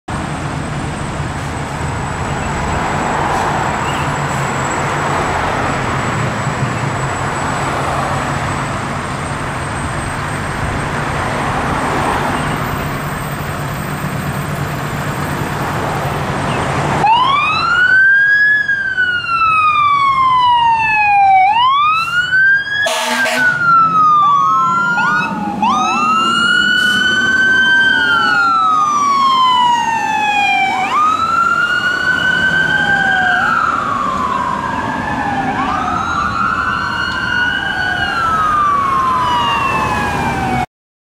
Toronto Fire Services - Pumper 445, Squad 445 & DC44 Responding.
Toronto Fire Services Station 445 dumping to a highrise fire alarm somewhere in West Toronto. Crews arrived to no signs and cleared the rest of the responding units.

Apparatus Info:
Pumper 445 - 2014 E-One Typhoon
Squad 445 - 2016 Spartan Gladiator Classic MFD Walk in Heavy Rescue
District Chief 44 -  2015 Chevrolet Tahoe